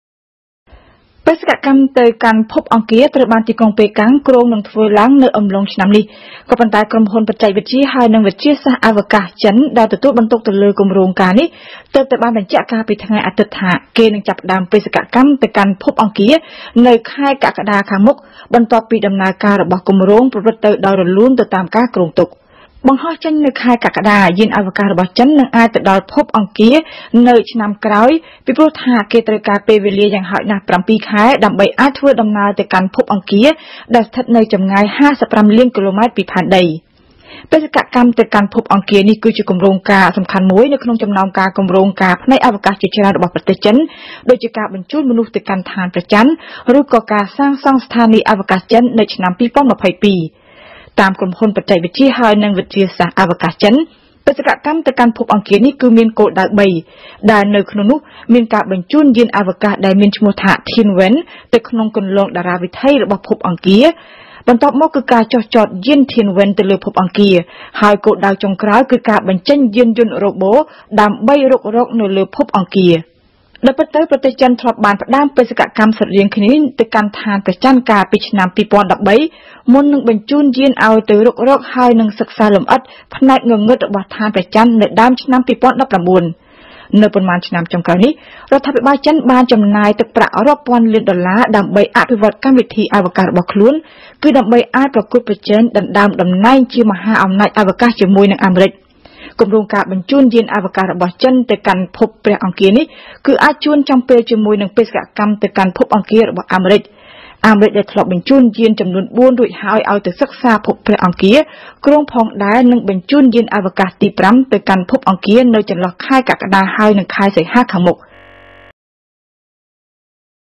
ចិន​គ្រោង​នឹង​បង្ហោះ​យាន​ទៅកាន់​ភពអង្គារ​នៅ​ខែ​កក្កដា ដើម្បីប្រជែងនឹងអាមេរិច
ប្រទេស​ចិន​នឹង​ចាប់ផ្តើម​បេសកកម្ម​ទៅកាន់​ភពអង្គារ នៅ​អំឡុង​ខែ​កក្កដា​ខាងមុខ។ នេះ​បើតាម​ការប្រកាស កាលពី​ថ្ងៃ​អាទិត្យ របស់​ក្រុមហ៊ុន​ចិន​ដែល​ទទួល​បន្ទុក​លើ​គម្រោងការ មួយនេះ។ ដំណឹង​នេះ កើតមានឡើង​នៅ​តែ ២០​ថ្ងៃ​ក្រោយ ដែល​ចិន​បាន​បង្ហោះ​ដោយ​ជោគជ័យ​យានអវកាស​ដែល​អាច​ដឹកមនុស្ស​ទៅកាន់​លំហអាកាស នៅក្នុង​មហិច្ឆតា​ប្រកួតប្រជែង​ជាមួយ​អាមេរិក ដើម្បី​ក្លាយជា​មហាអំណាច​អវកាស។